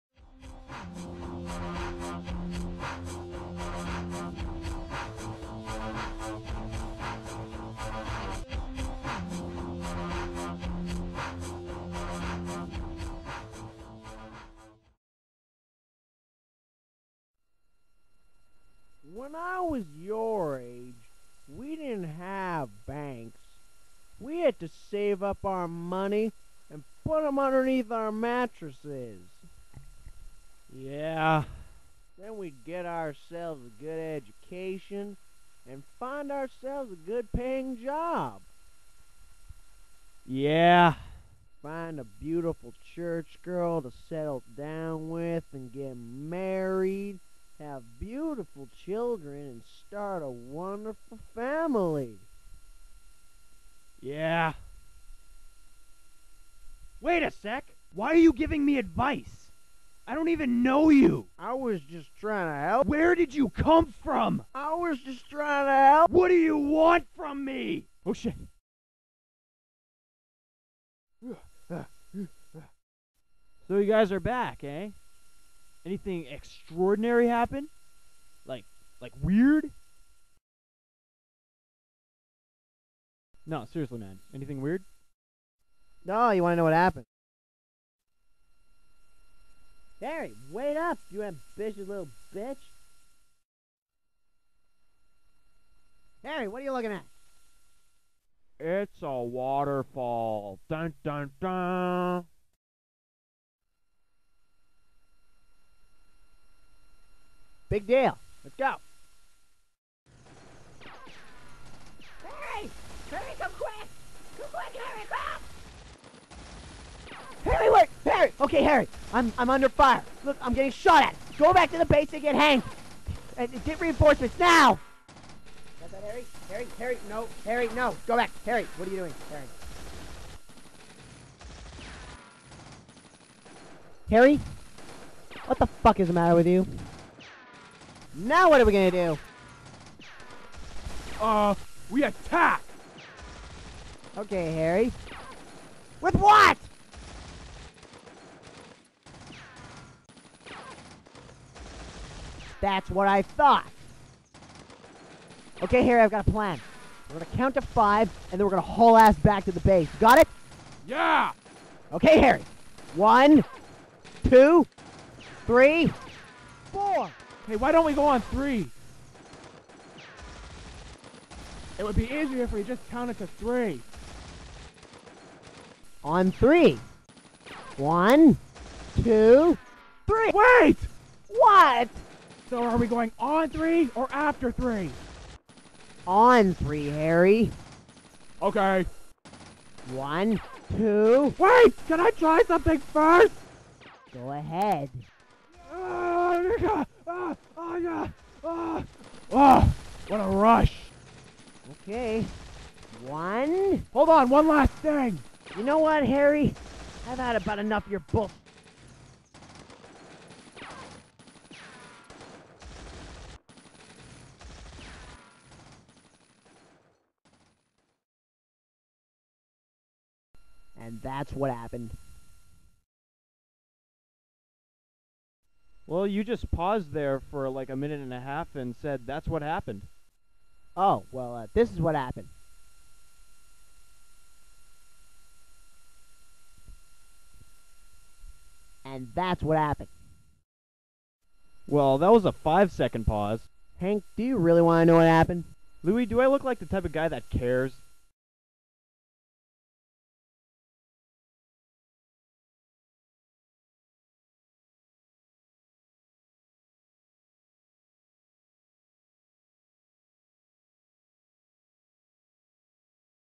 Us vs. Them: The Machines Within - Ep.02 - The Sequel
This unscripted show was made back in 2003. Mostdramatic is surprised with itself for not uploading this when You Tube was made, or even till mid 2009 came around. This is one of Mostdramatic's favorite productions. It was their first attempt at using machinima as their medium. Great fun to make, though very stressful at times. Mike, Corban and Jared just picked their characters and winged it. Don't take it too seriously, they didn't. Enjoy!